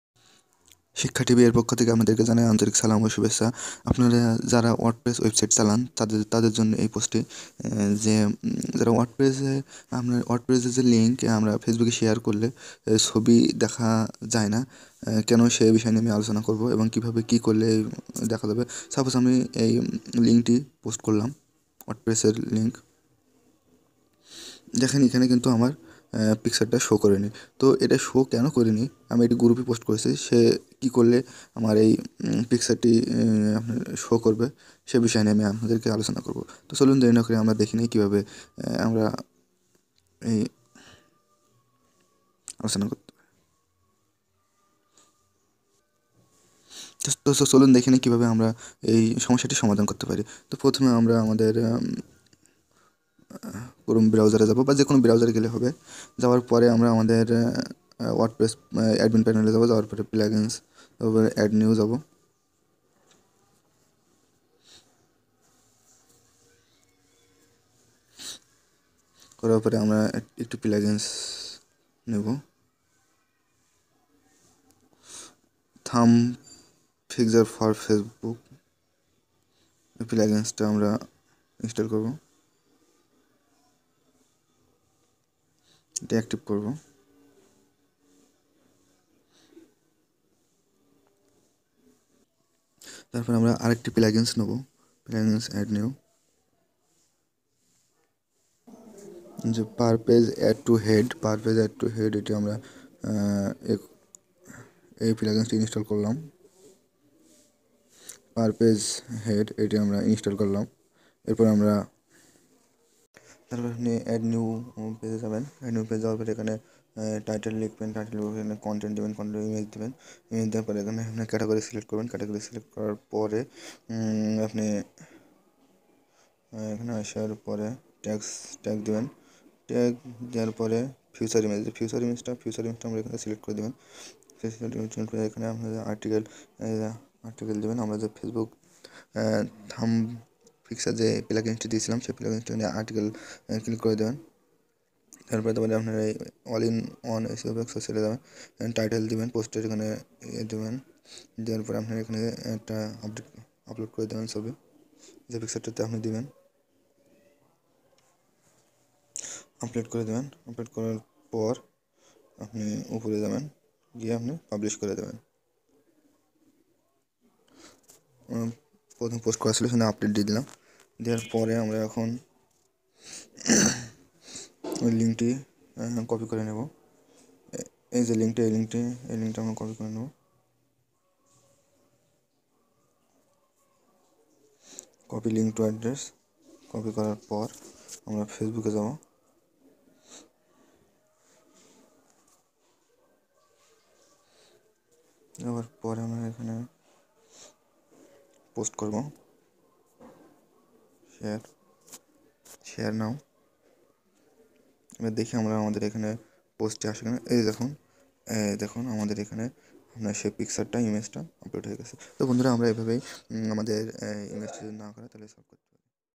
Wordpress featured image isn't showing to facebook.
Wordpress featured image facebook এ দ্যাখা জাচ্ছেনা নিন সামাধান।